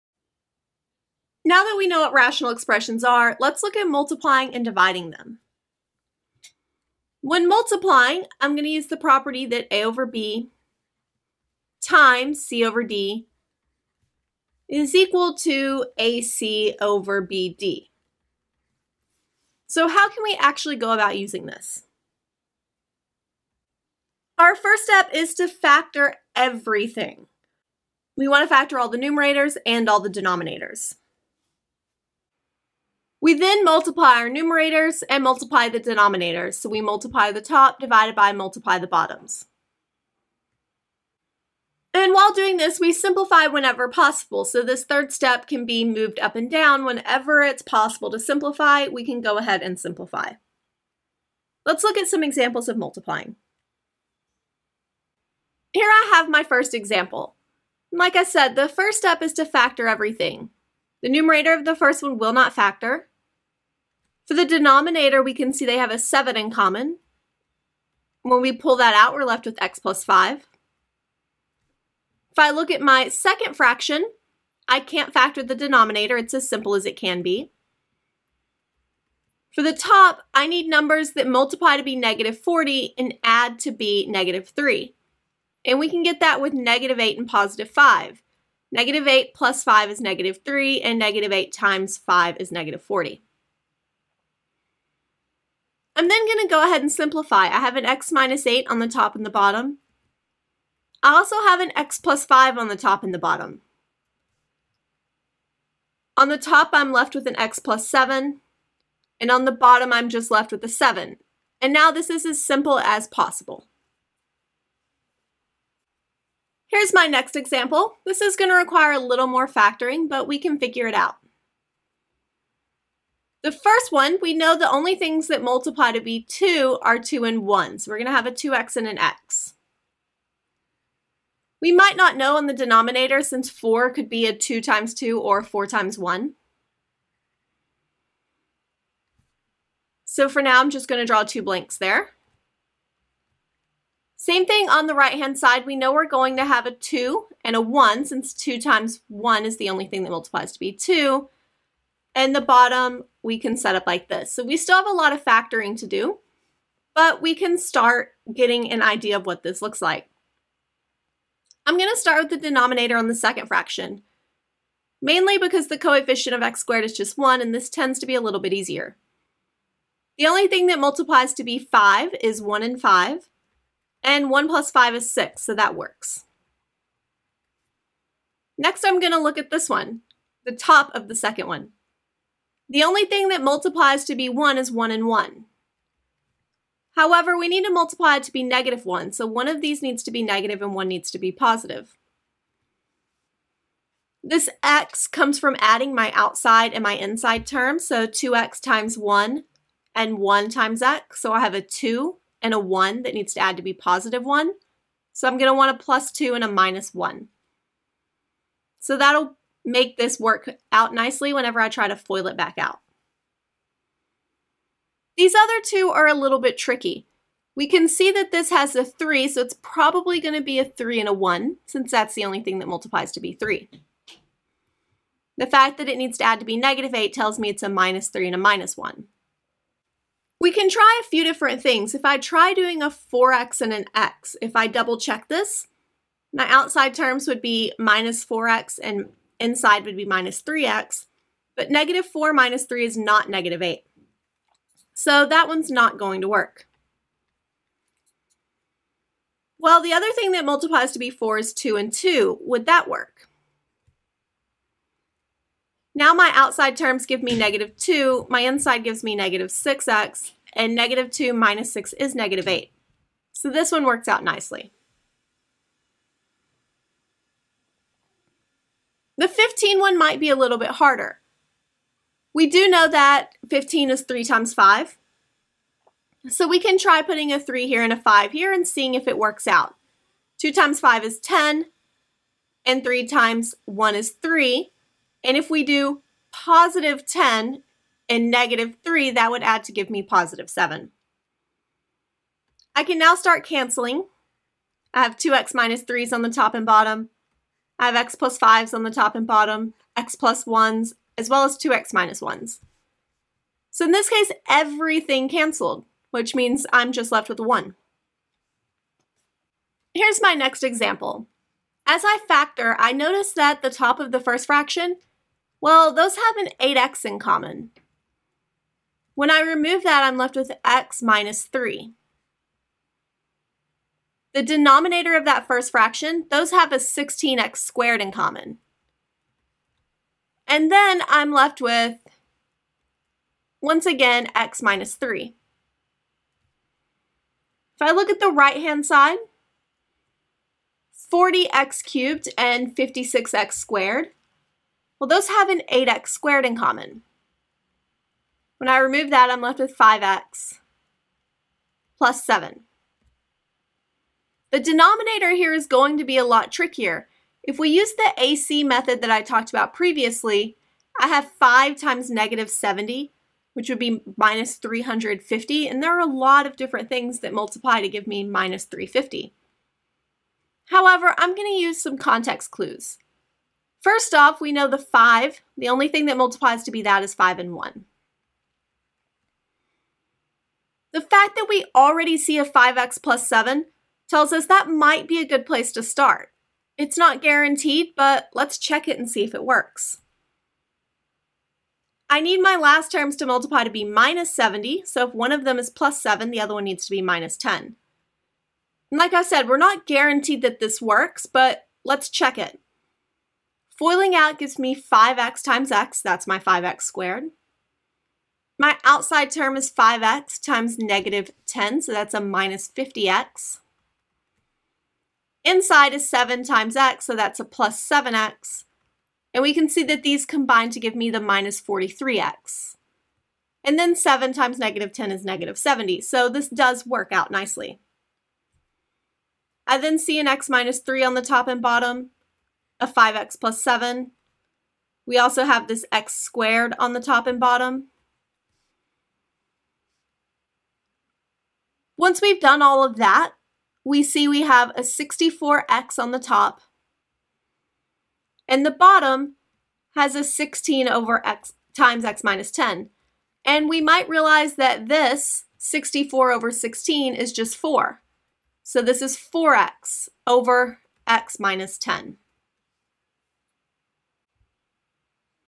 Multiplying Rationals
I discuss how to multiply rational expressions.